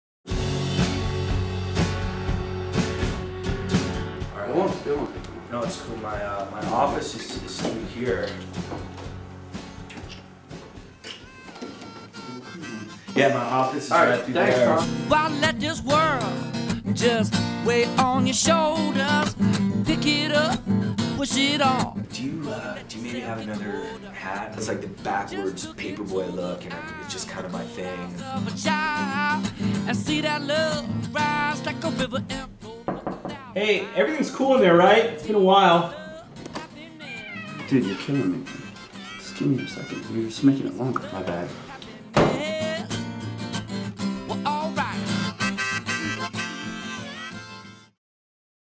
Music and Words G. Love Trailer.mov
G. Love appears on my show and blows my socks off!